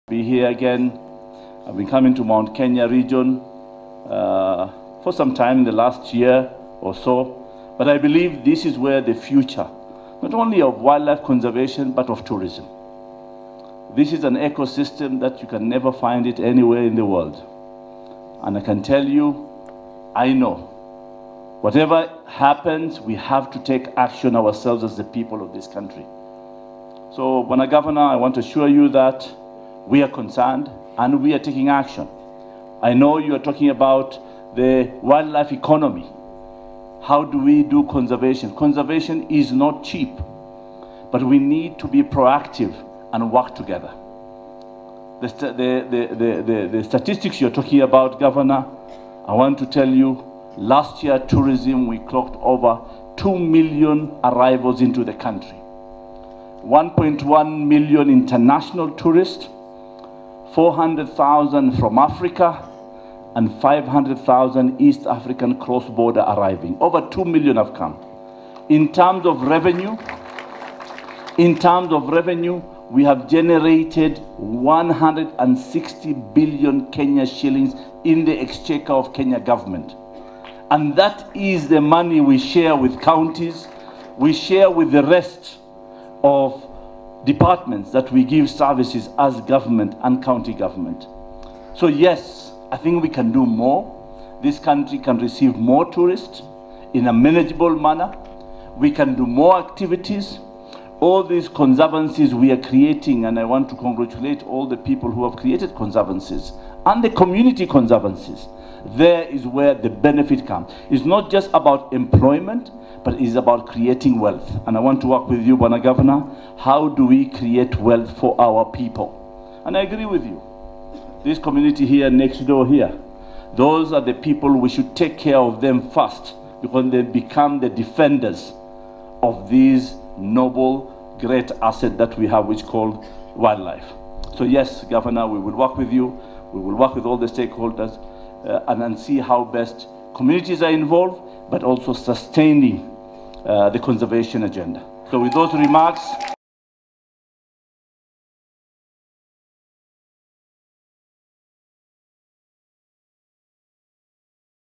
Balala launches the national recovery for mountain bongo at Mt Kenya Wildlife Conservancy
Tourism and Wildlife Cabinet Secretary Najib Balala speaking on the status of tourism in the country.
He addressed wildlife stakeholders when launching the national recovery and action plan for mountain bongo (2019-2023) at Mount Kenya Wildlife Conservancy in Nanyuki.